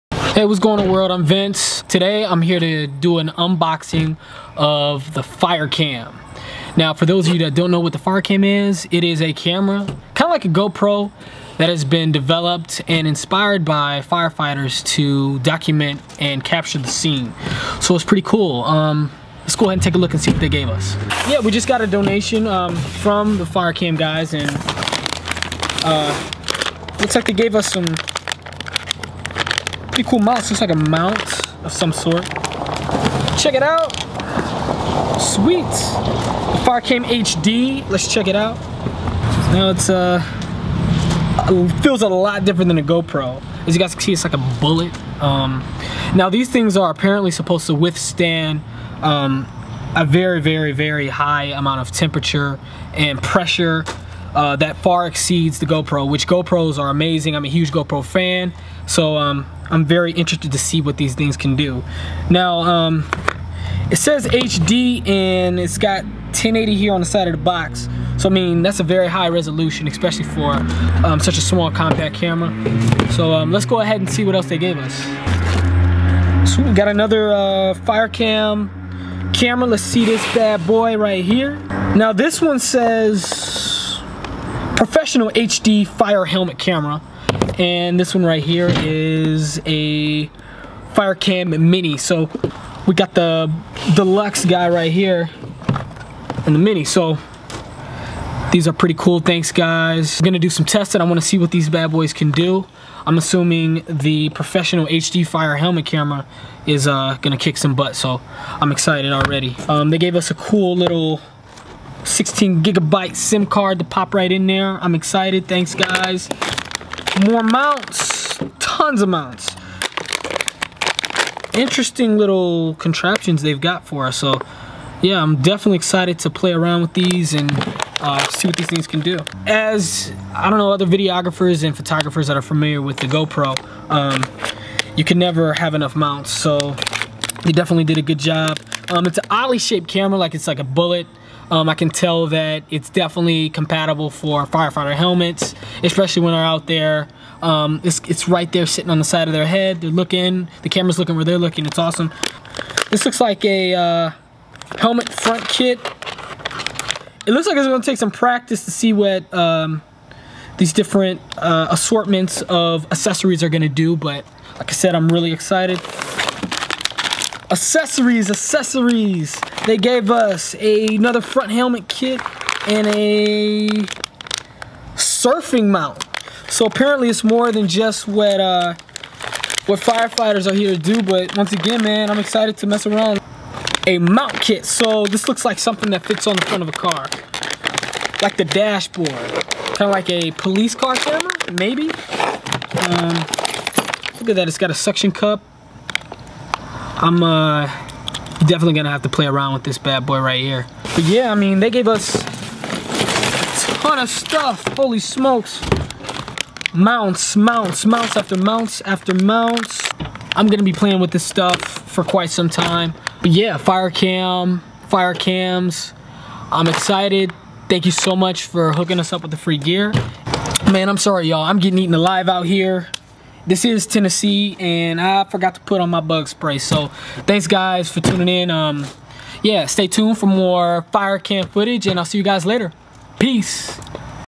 Fire Cam Unboxing
Flip Fest received the new "Fire Cam," here is the unboxing of the FIre Cam and accessories